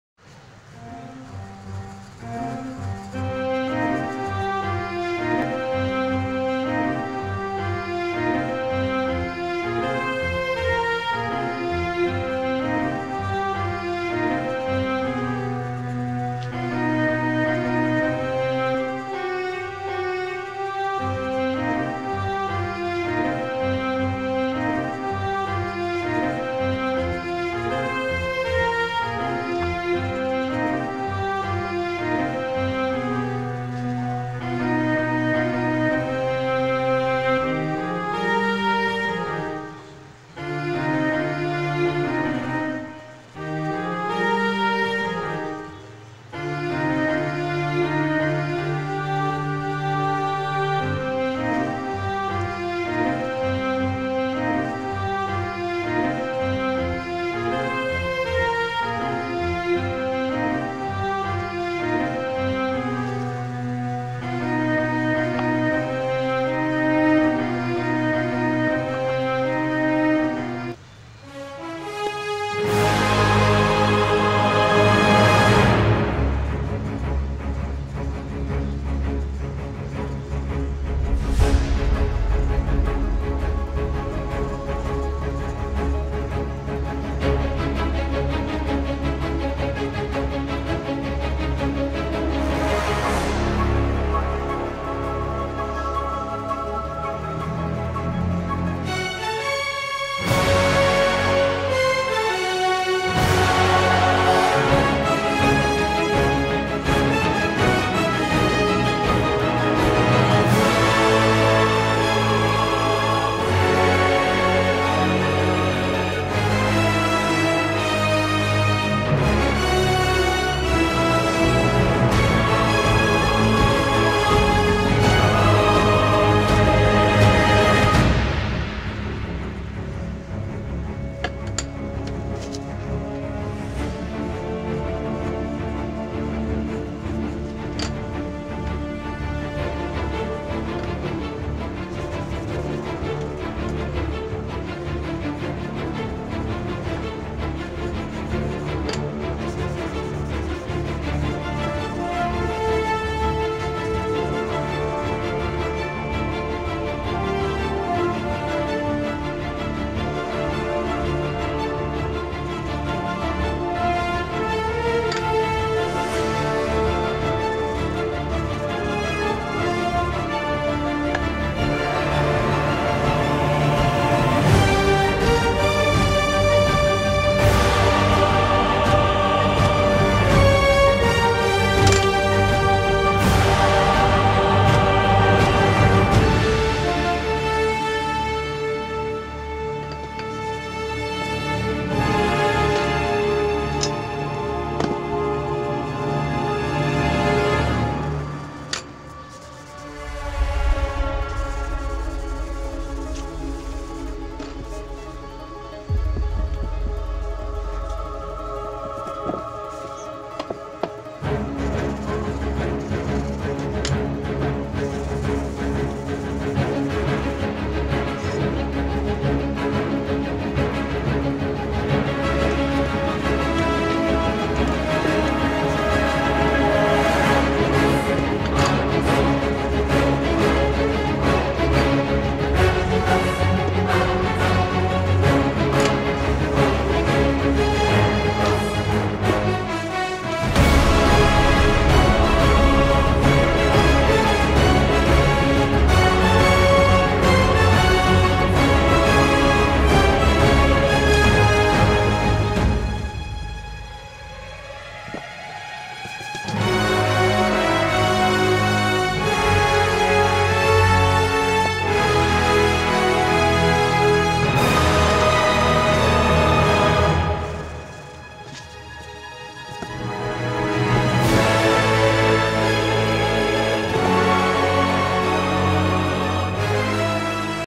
HOW TO DRAW: MYSTERIO (From Spider-man Far From Home)
Well this is like my billionth Mysterio Drawing :/